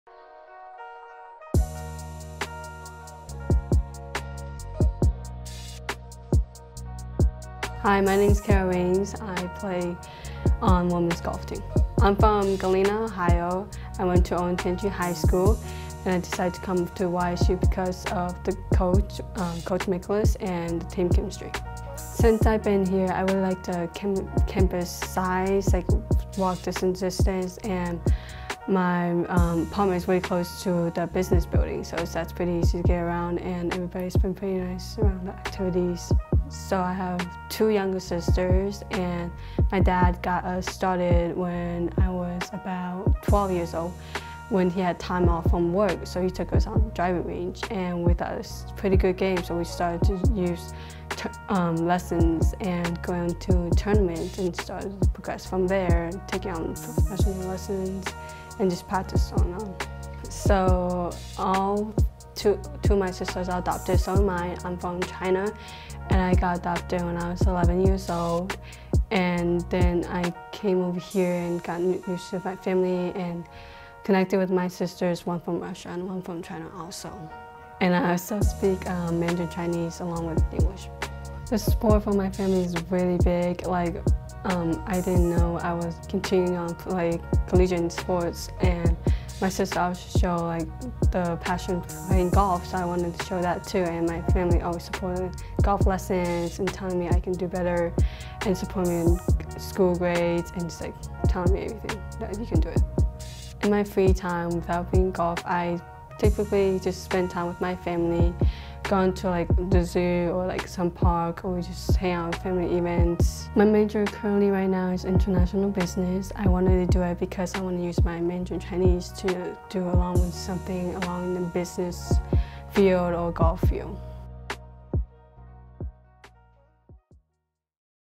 Get to Know | Kara Raines | YSU Women's Golf
We get to know Kara Raines of the Youngstown State women's golf team. Kara is an International Business major and a native of Galena, Ohio. She was named a WGCA All-American Scholar as a freshman in 2017-18.